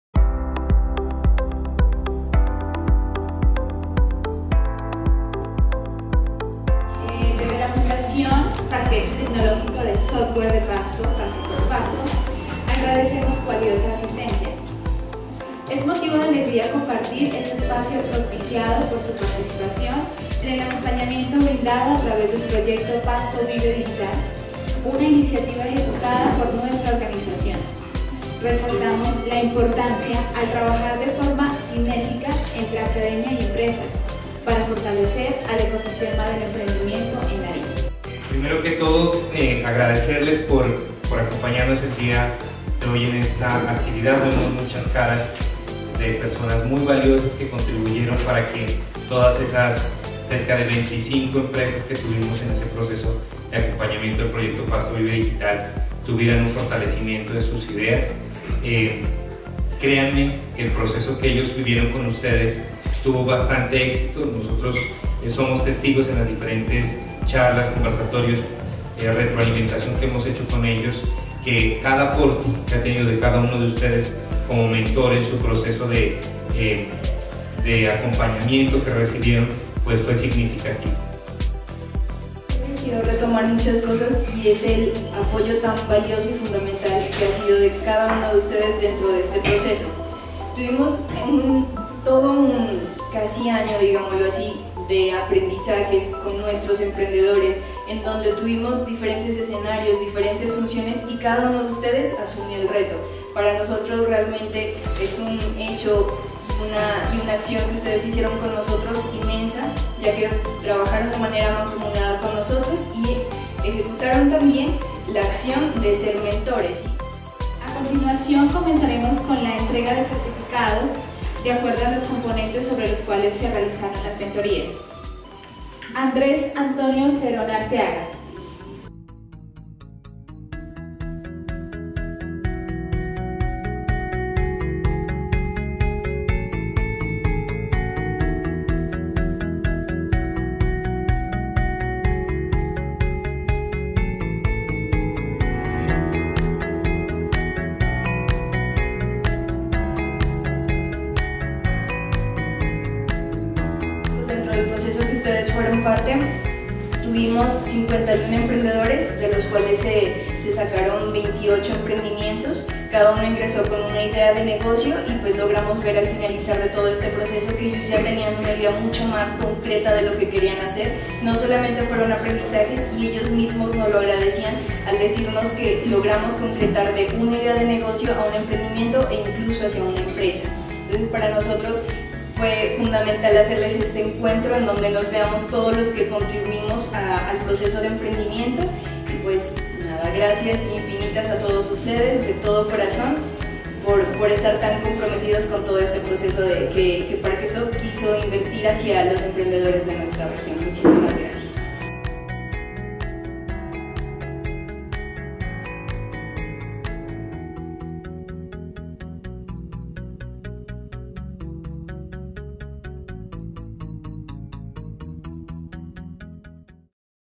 Experiencias que inspiran a emprendedores, ¡Gracias Mentores!
ParqueSoft Pasto en alianza con la Institución Universitaria CESMAG, Universidad Autónoma de Nariño promueven el fomento al emprendimiento en la región, labor por la cual recibieron certificación por el apoyo y procesos de mentoría en el marco de ejecución del Pasto Vive Digital mismo que contribuyó al desarrollo y fortalecimiento de la industria empresarial en el municipio.